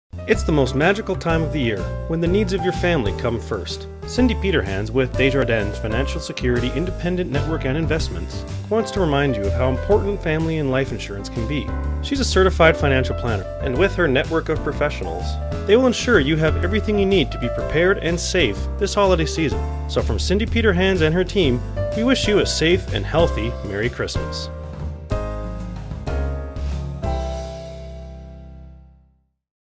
Cindy Peterhans "Christmas Greetings" - Cartoon Video
An animated Christmas cartoon we made for Cindy Peterhans with Desjardins Financial Security Independent Network.